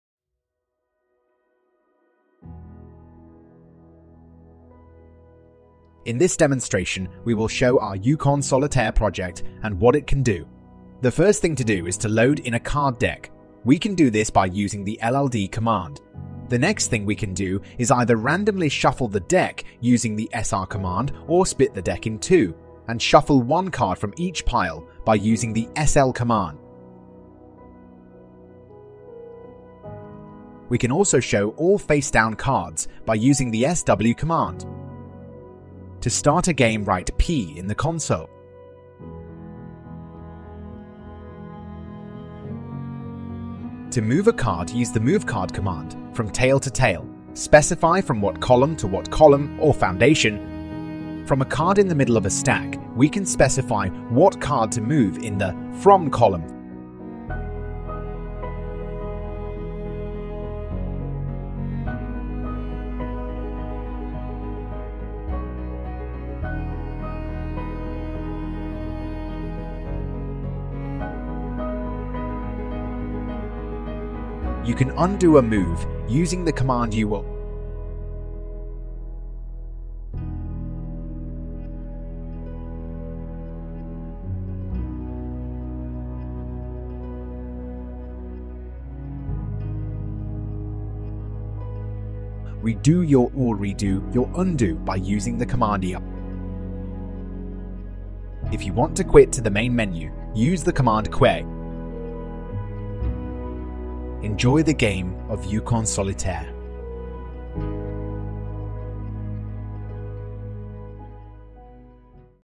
Group 28 - Yukon Solitaire in C
Project from DTU course: Machine Oriented Programming, Fall 23.